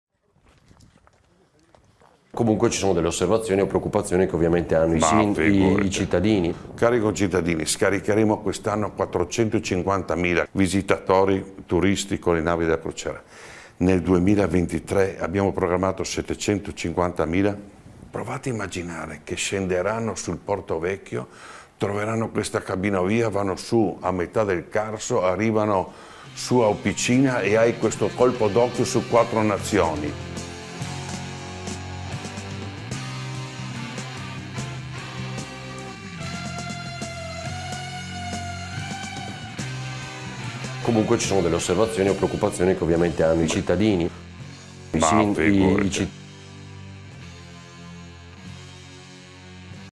Cabinovia di Trieste, lo striscione di protesta gigante srotolato in Carso
"Oggi qualcun* ha deciso di esprimere la sua contrarietà allo stupido e dannoso progetto dell'ovovia, proprio nell'ambiente che verrebbe distrutto dalla sua costruzione. Ci vediamo lunedì 5 dicembre davanti al Comune di Trieste alle 18.30, per difendere dalla devastazione il territorio in cui viviamo!" Sono le parole che accompagnano il video che mostra alcuni attivisti contro la Cabinovia srotolare uno striscione gigante in Carso. Tutto questo alla vigilia della seduta straordinaria del Consiglio comunale convocato domani (lunedì, appunto) sul tema. L'articolo